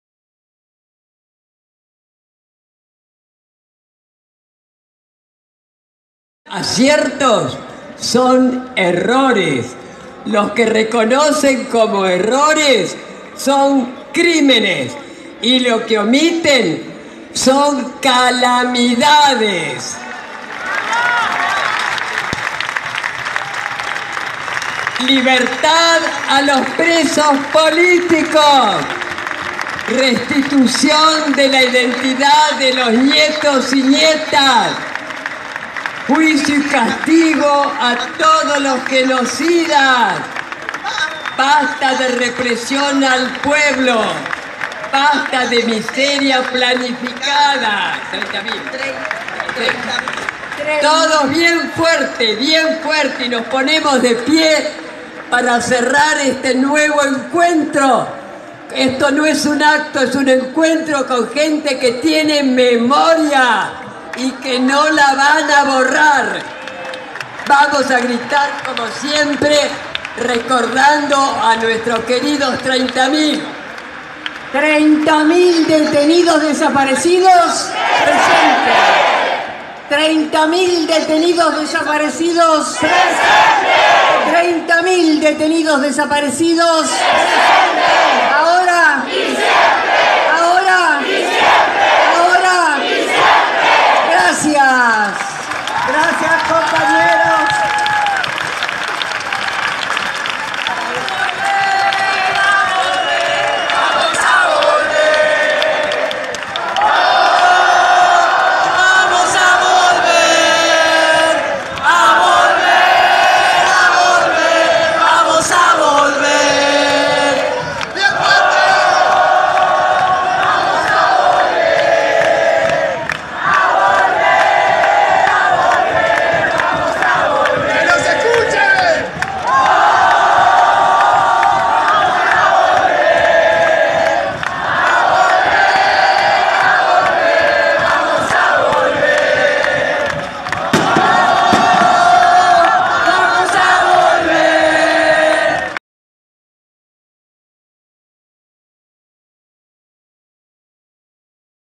Homenaje de la Tevepé a las Madres de Plaza de Mayo
40 años de lucha y resistencia! Grabado ayer 24 de marzo de 2017 en la Plaza de Mayo